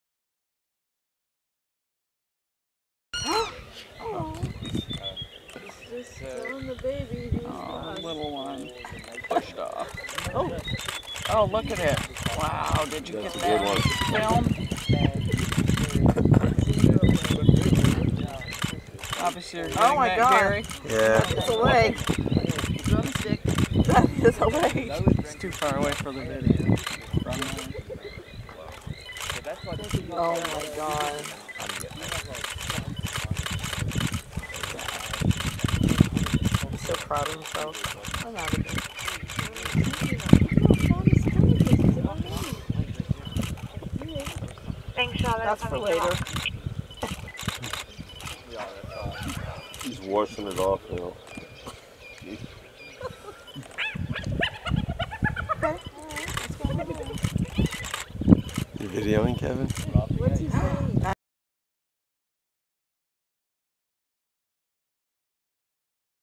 Hyenas steal lion kill
A huge clan of hyenas stole a zebra kill from a pride of lions.